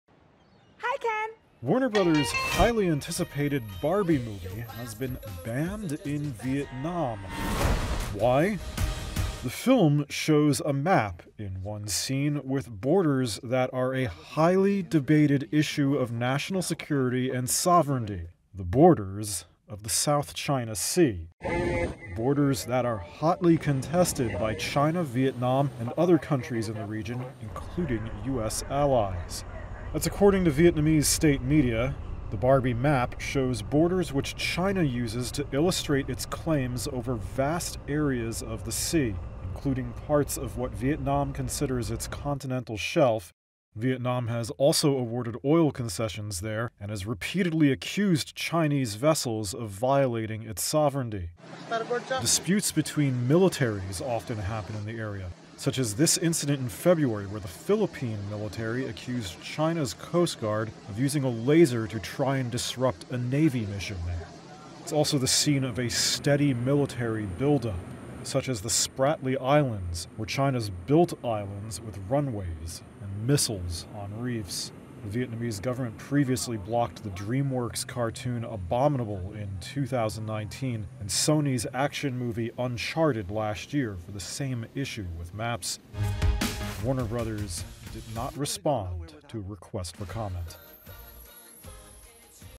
Vietnam bans the Barbie movie
Vietnam's government has blocked domestic distribution of Warner Bros' highly-anticipated film, "Barbie." The problem is a scene which features a map of the South China Sea, whose borders have long-raised fierce national security and sovereignty debates across the region. Matthew Larotonda reports.